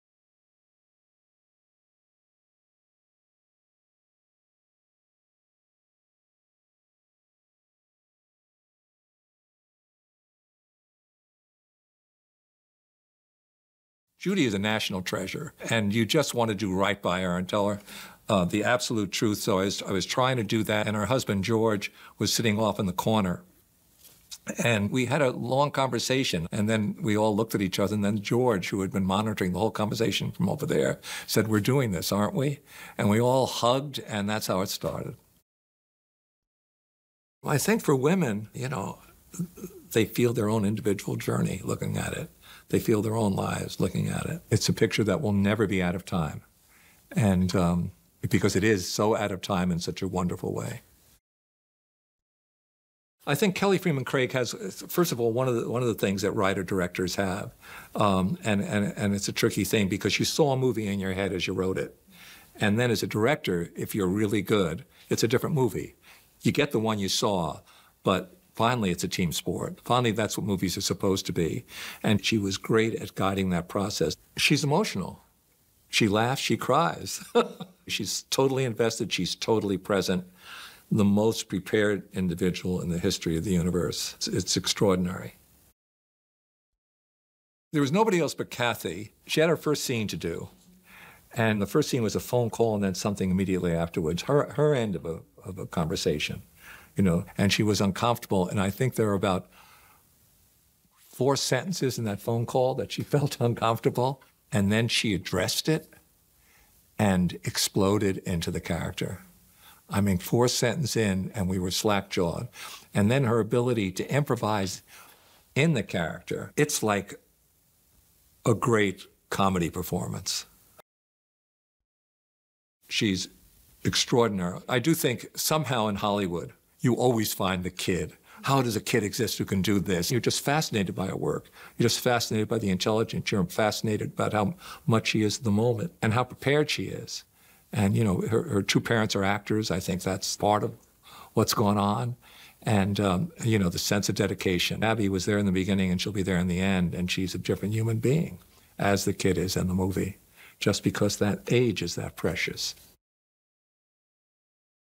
Are You There God? It’s Me, Margaret - James L. Brooks - Producer | Interview
Raised by a Christian mother and a Jewish father, an adolescent girl starts to ask questions about faith and religion.
Release date: April 28, 2023 (USA)
Director: Kelly Fremon Craig
Screenplay: Kelly Fremon Craig
Distributed by: Lionsgate Films
Adapted from: Are You There God? It's Me, Margaret.